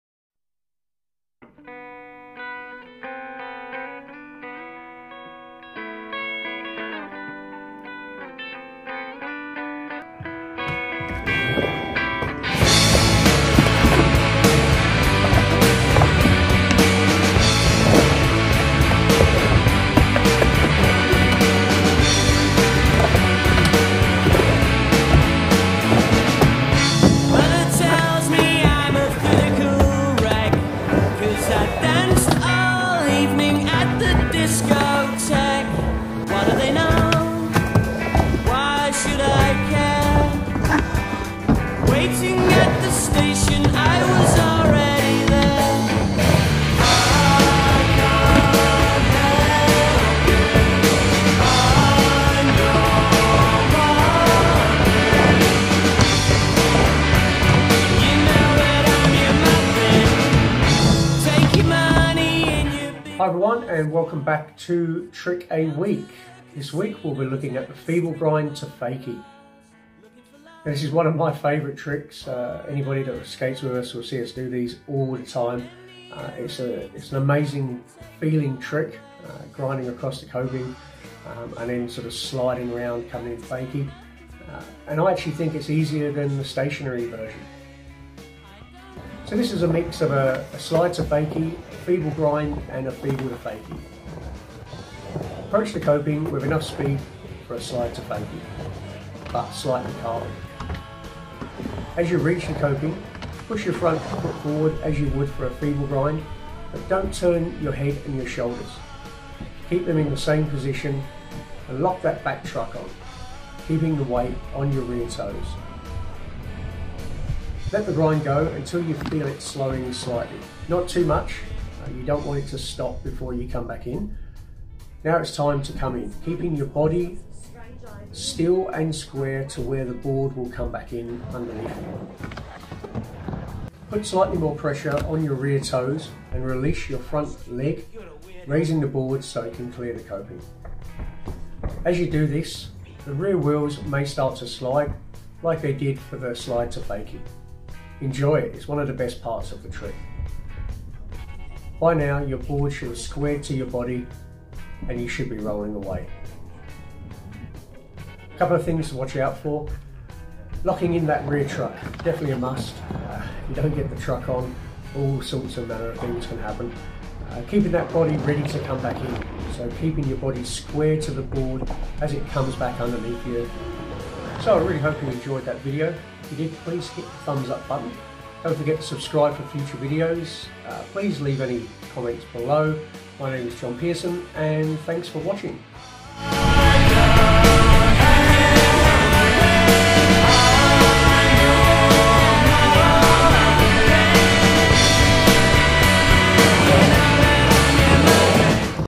week 25, Trick a Week, Feeble grind fakie. learning to skateboard on vert 40+
Hi everyone and welcome back to trick a week.
this week the feeble grind to fakie.
Now this is one of my favourite tricks so I really hope you enjoy the video.